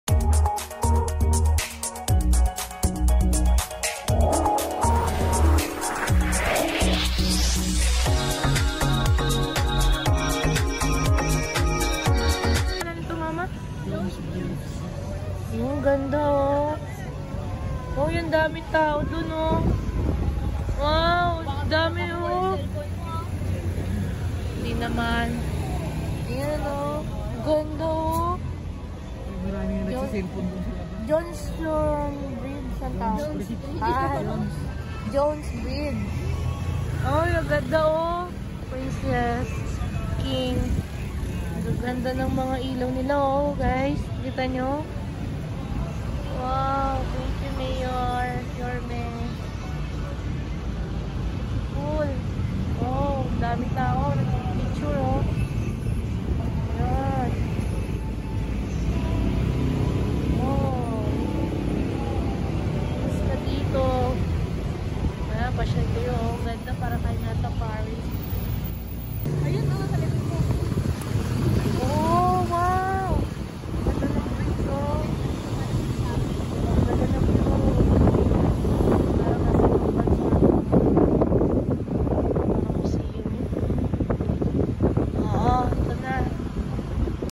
JONES BRIDGE MANILA SIKAT NGAYON 😍♥️Pasyalan
Jones Bridge Isa sa sikat ngayong pasyalan dito sa Binondo Manila Super clean and beautiful place to pic and pasyal 😍tara na mga 💕KaBangsy